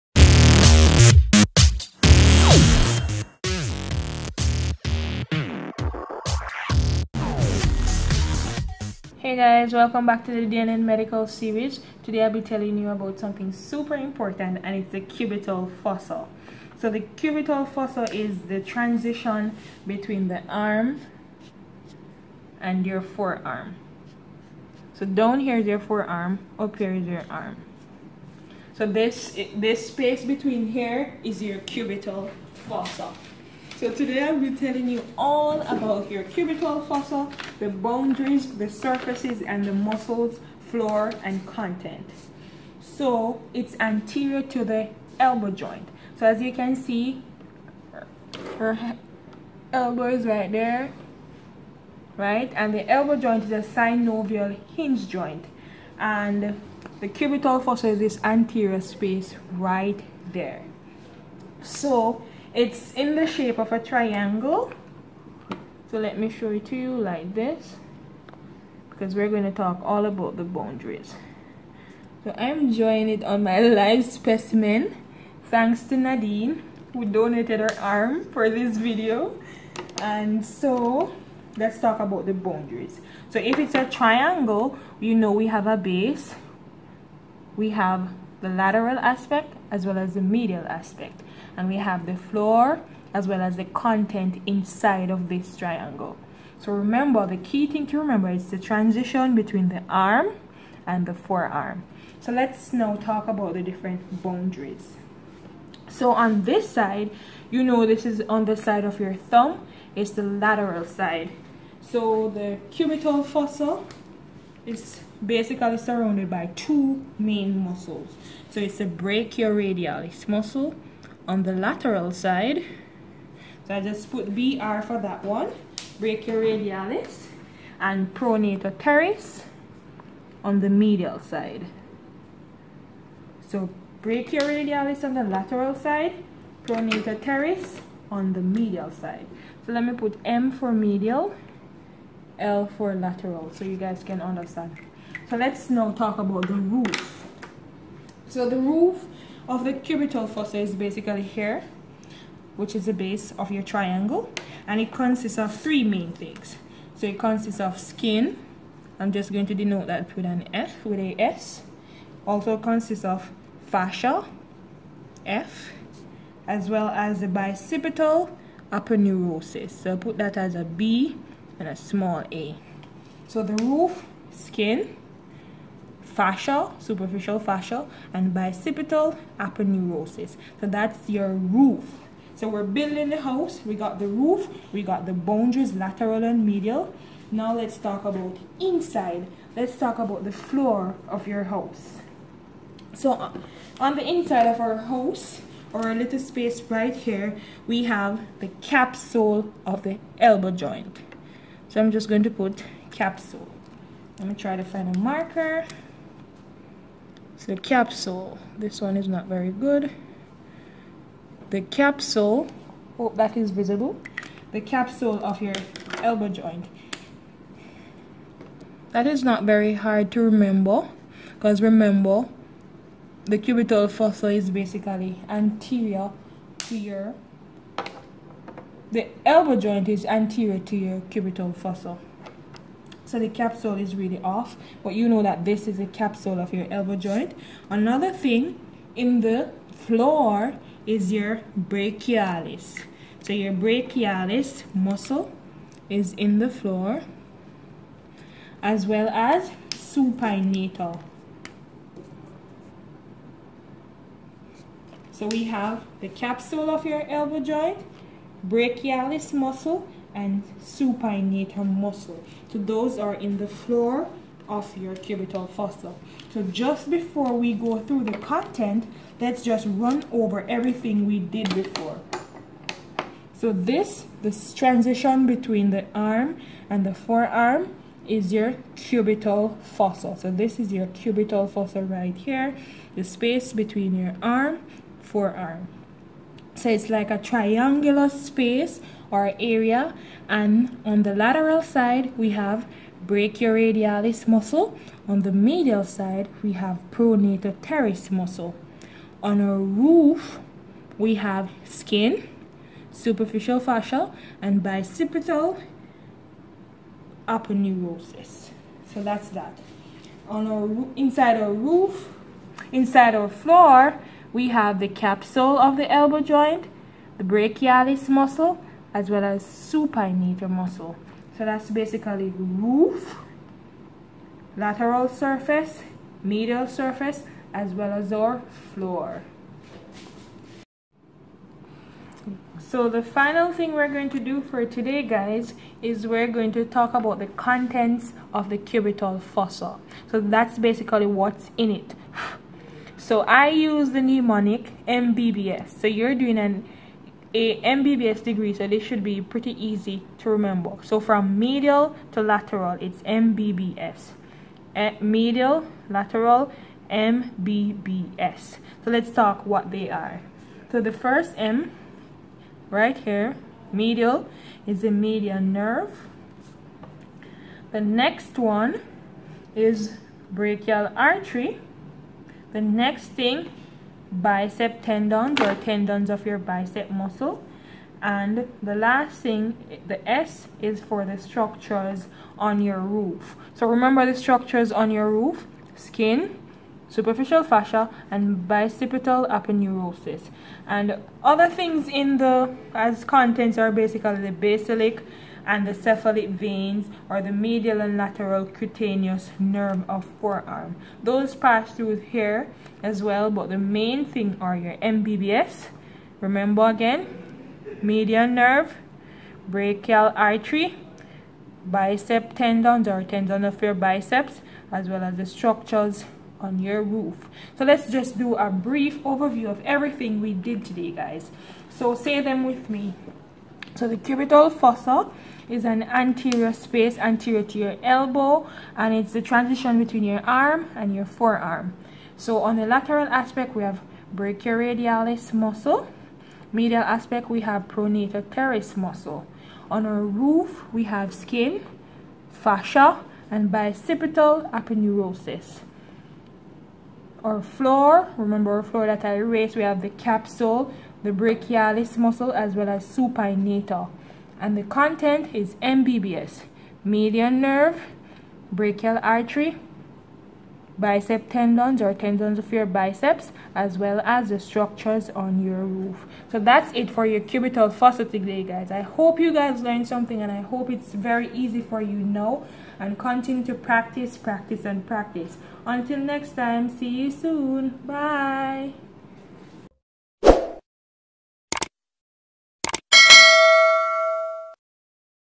CUBITAL FOSSA: EASY WAY TO REMEMBER IT: D&N Medical Series
This video provides a quick and easy way to remember the Cubital Fossa, its content and its boundaries. Tell us any other techniques you use to remember it, in the comment section below.
Correction: The cubital fossa is anterior to the elbow joint and not the other way around, as wrongly said in a short part of the video.